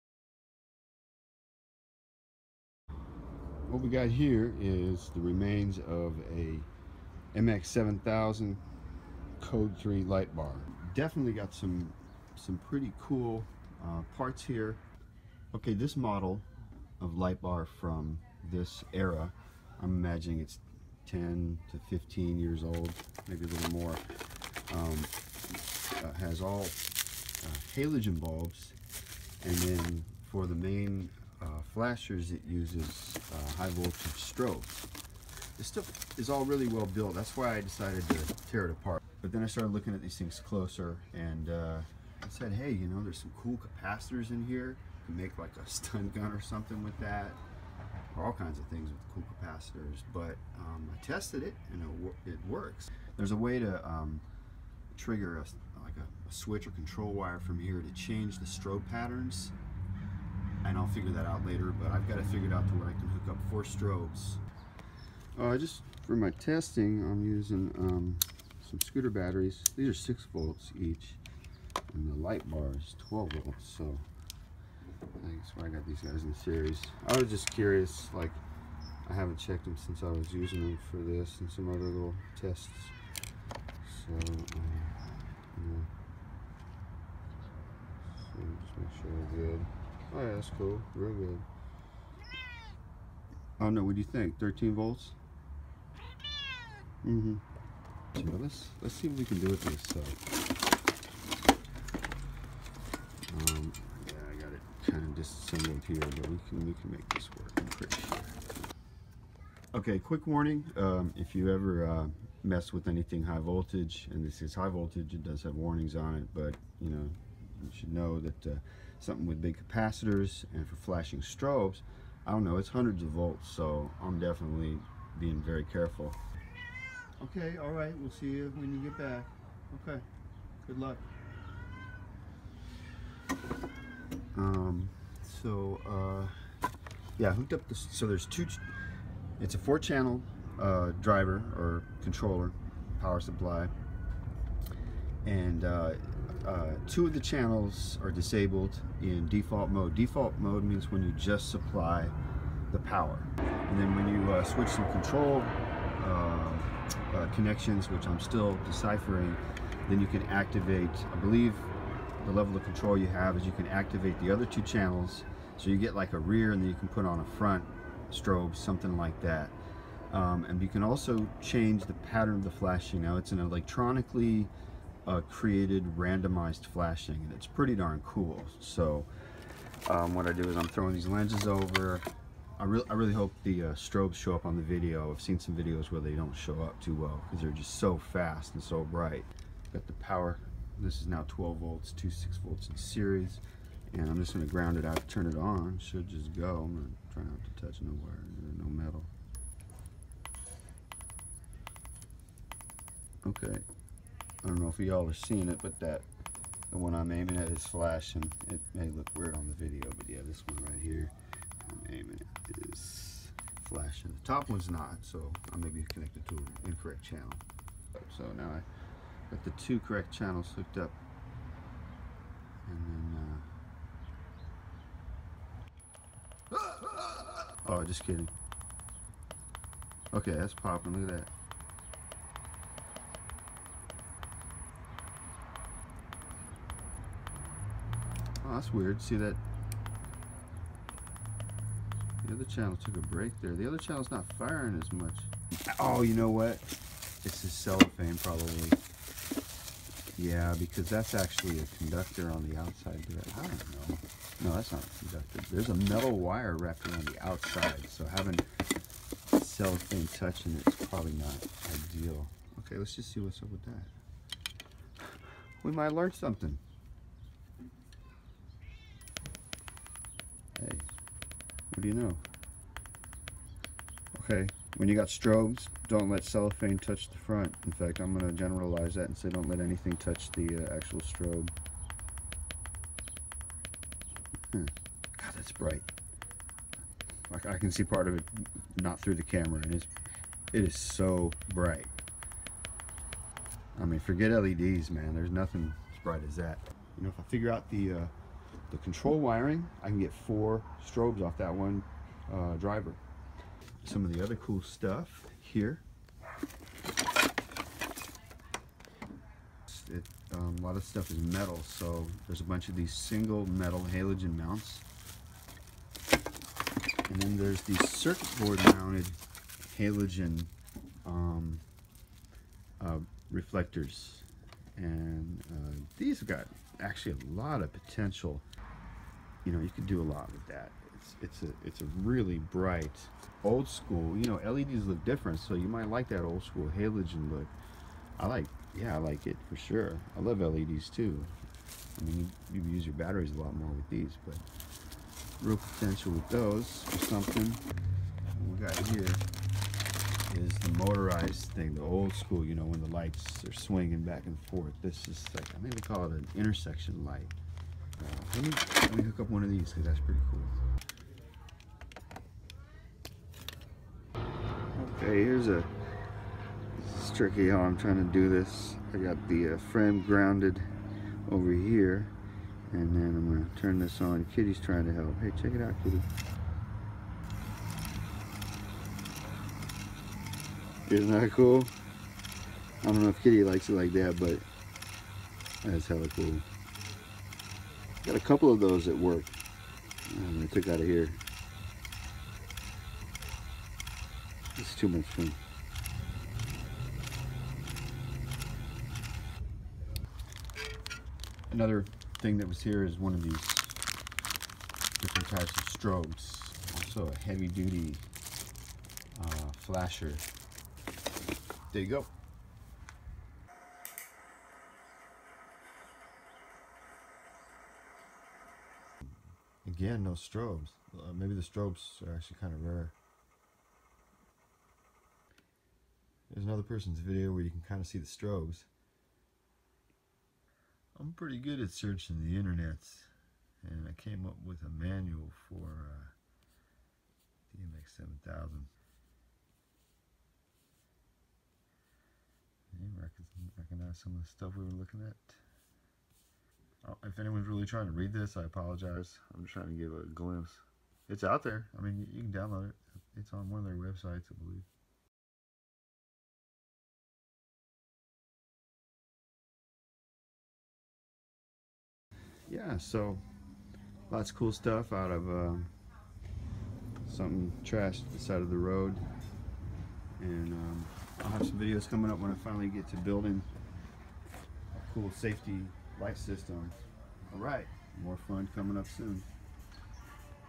Salvaging Strobes + Controller from an Emergency Vehicle Light Bar - Code3 MX7000
A smashed up tow-truck light bar from about 2000 yields high-end strobs and driver circuitry, motorizd lights and more. Learn about the MX7000 and it's strobe controller units. If you think you have some bright LED lights or bright LED strobes, you need to look at some real old school strobes. Much brighter. And the look of mechanical oscillating lights is really cool and something you don't see anymore. My plan is to build custom safety lights for my main working tractor and my tractor-hauling trailer since both are used on a dangerous road and I will really benefit from the very high visibility of these ultra-bright strobe flashers and their attention-grabbing random flashing pattern.